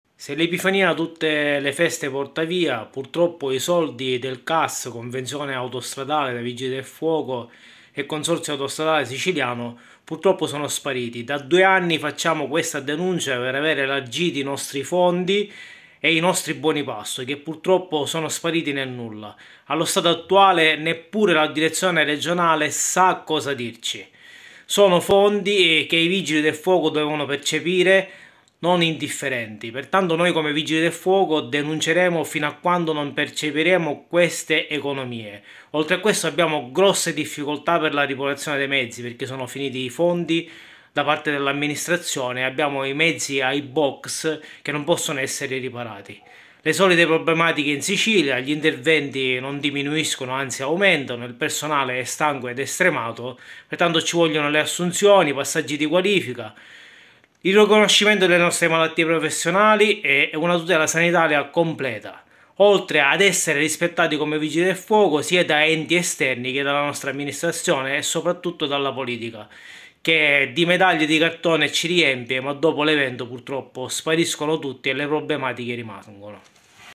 Catania. Vigili del fuoco : ancora nulla dei pagamenti convenzione Cas
Vigili del fuoco, Sicilia: ancora nulla dei pagamenti  Cas autostrade, la denuncia di Carmelo  Barbagallo usb vigili del fuoco Sicilia.